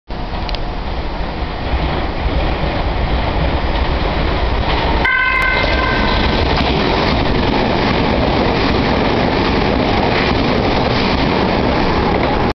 名鉄大江駅 2200系 (2201F 特急岐阜ゆき) 通過
特急 岐阜ゆき